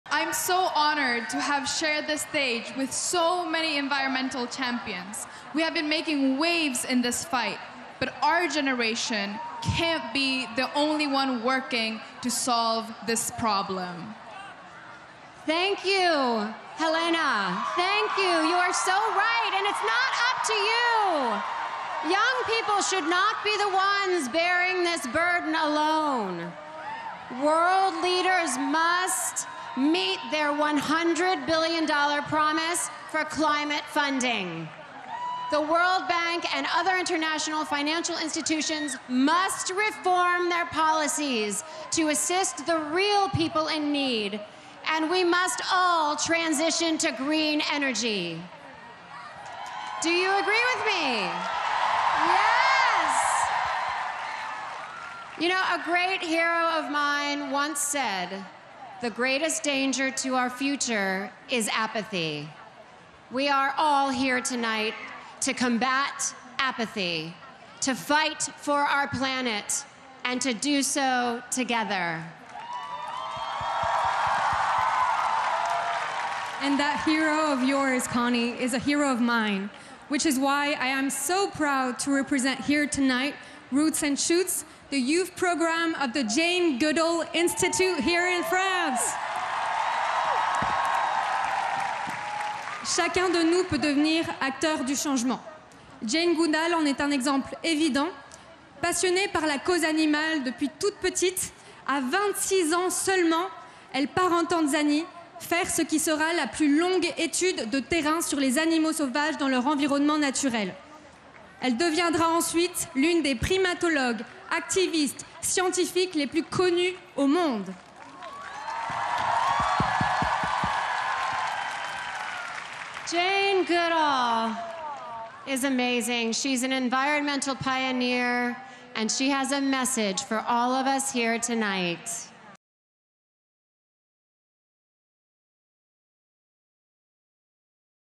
Helena Gualinga, Constance Cordier & Connie Britton Combat Apathy | Power Our Planet: Live in Paris
Climate activist Helena Gualinga joins Constance Cordier, from the Jane Goodall Institute in France, and actress Connie Britton on stage at Power Our Planet: Live in Paris, on June 22, 2023, to highlight that it can't just be young people fighting for climate action and that every generation and every single one of us — from world leaders and heads of world banks, to every individual around the world — has an important part to play in fighting for our planet.